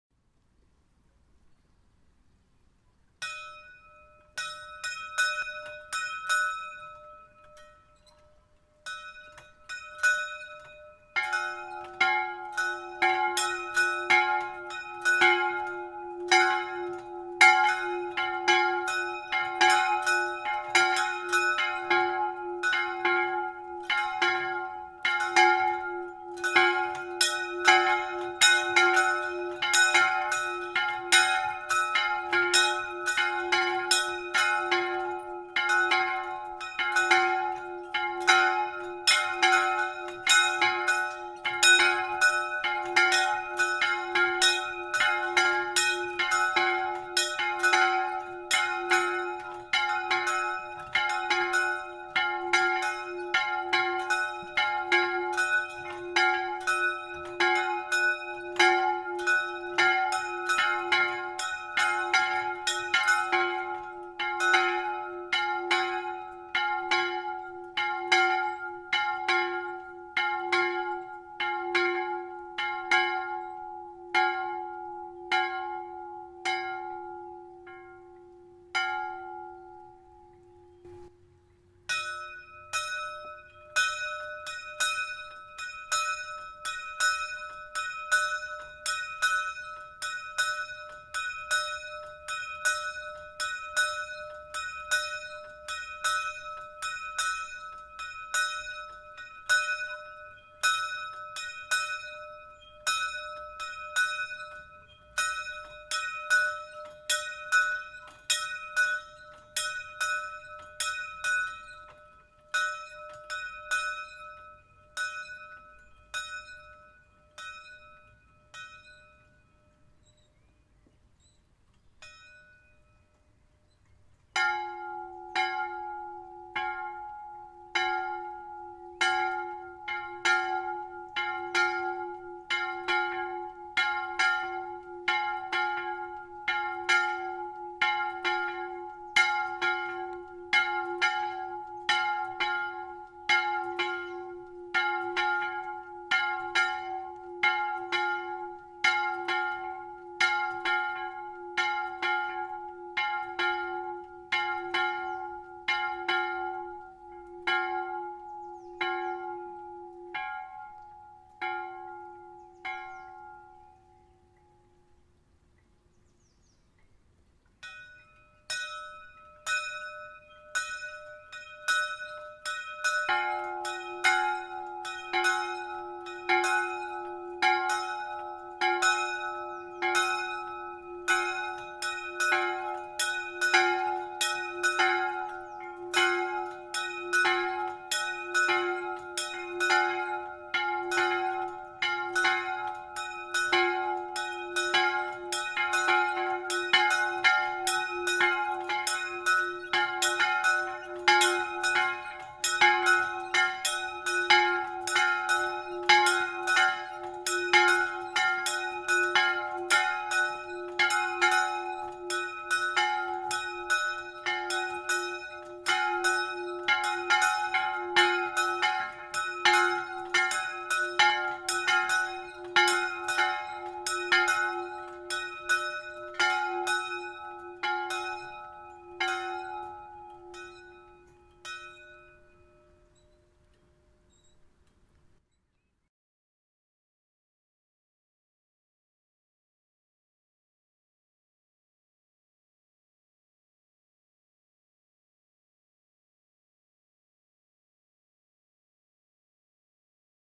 Ramstahl (KG) Die historischen Glocke der Marienkapelle auf dem Berg (Handläuten)
Etwas oberhalb von Ramsthal erhebt sich die Marienkapelle. In ihrem Turm befinden sich zwei historische Glocken, die von Hand gezogen werden. Die große Glocke klingt etwas blechern, während die kleine Glocke sich zimbelhell entfaltet. Zusammen ist es ein einzigartiges Duett mit Wiedererkennungswert.

1 | Große Glocke | es''
2 | Kleine Glocke | f'''

Die Ramsthaler Marienkapelle wurde bereits 1888 erbaut, da ein Mann weil er hier mehrmals betete von seinem Nervenleiden geheilt wurde. 1939,1940 und 1942 wurde die Kapelle renoviert. 1990 und 1998 wurde sie nochmals umgestaltet und ist nun ein Schmuckstück für Ramsthal!